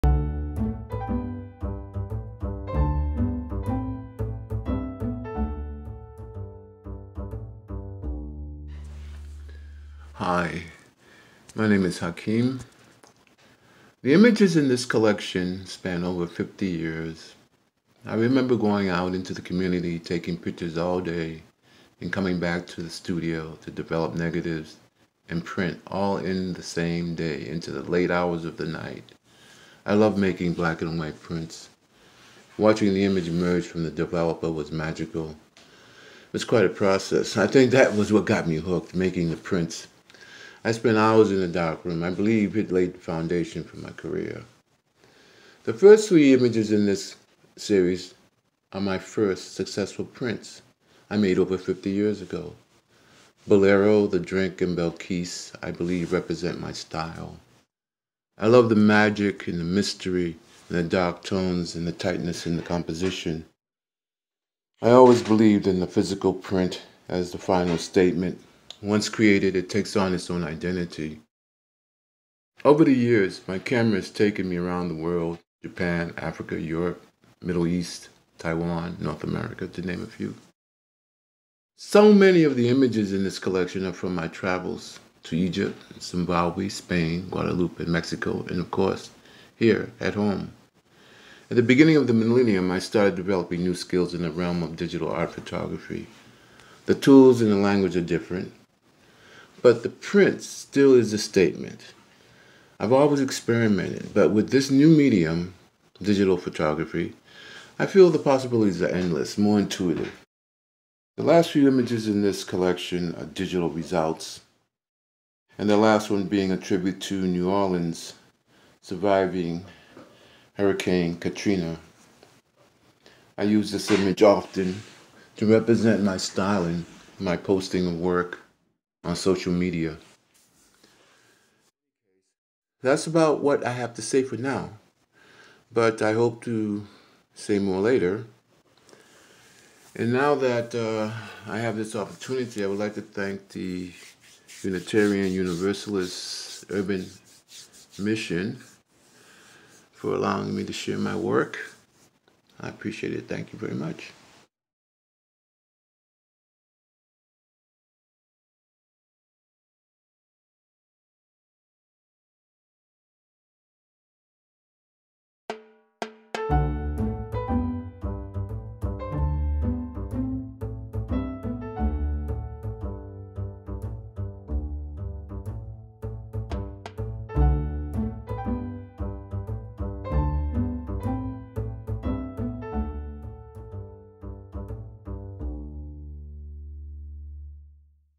Artist Spotlight: Hakim Raquib
This video is the first in a 3-part series featuring photographer Hakim Raquib. This video is a virtual gallery with Hakim's narration.

Video editing by Bithyah Israel.